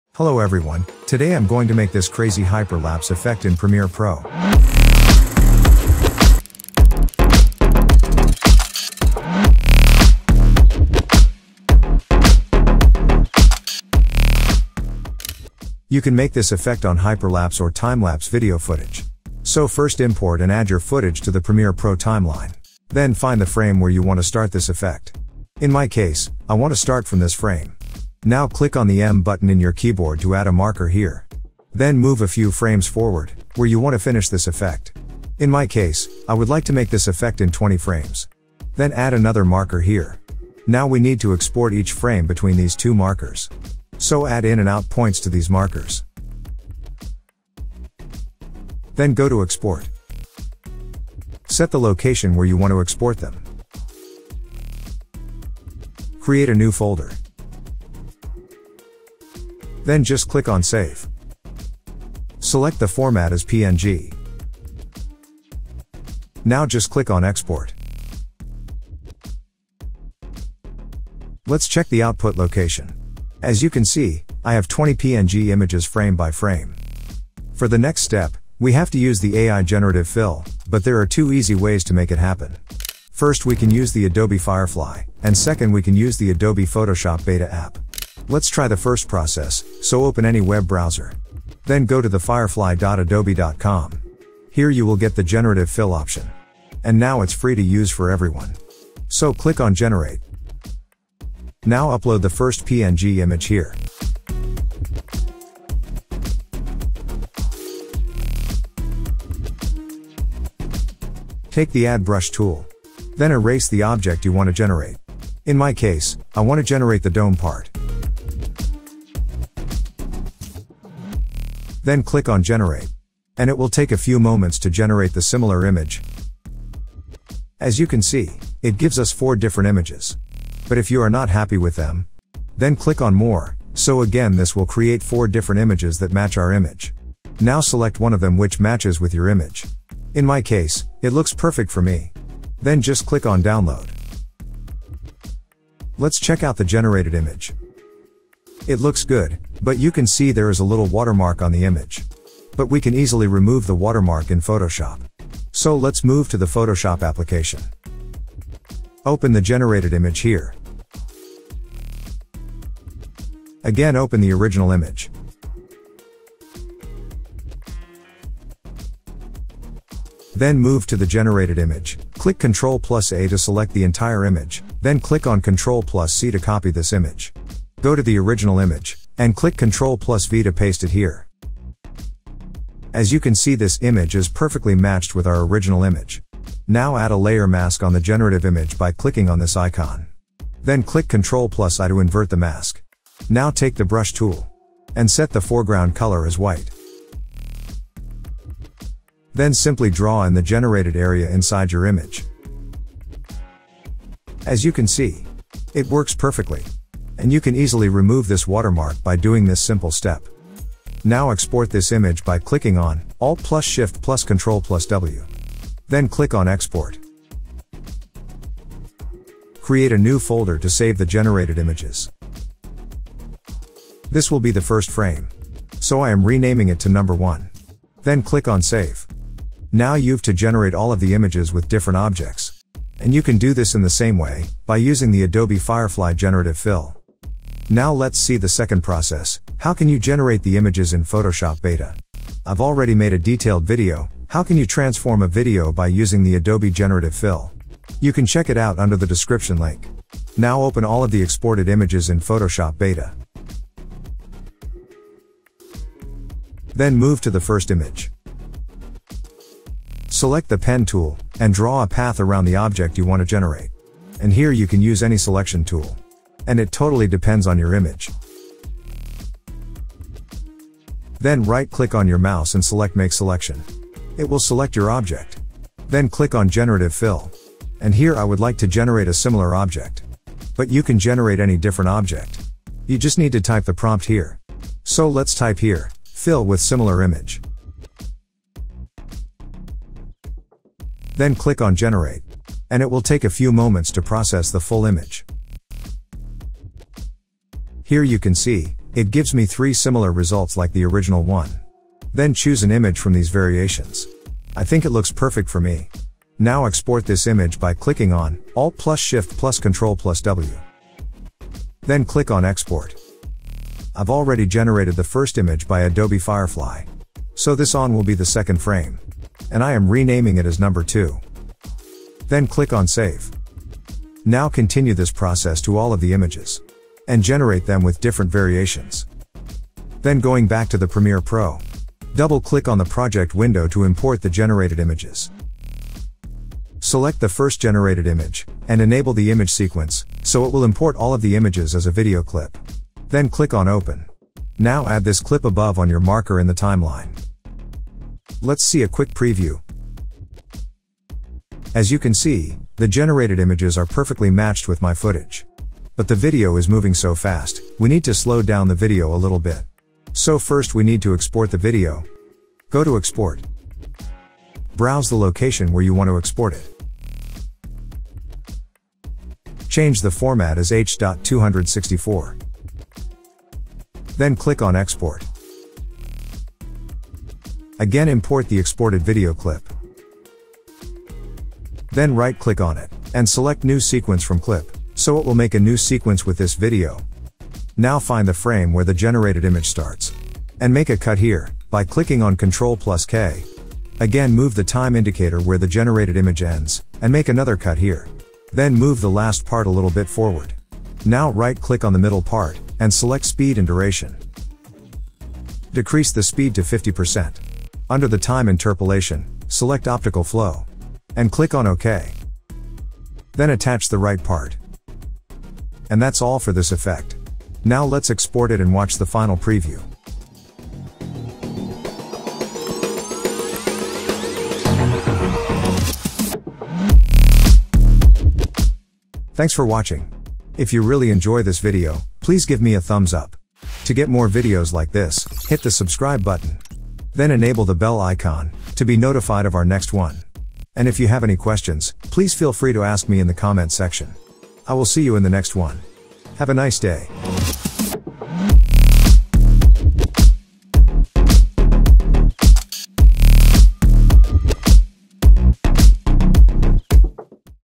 AI HYPERLAPSE Animation Effect Tutorial in Premiere Pro | Generative Fill
In this video, we are going to Make AI HYPERLAPSE Animation Effect Tutorial in Premiere Pro. This effect can be used to create a futuristic or amazing-looking animation.

To make this effect, we are going to use the Generative Fill feature in Adobe Firefly & Adobe Photoshop beta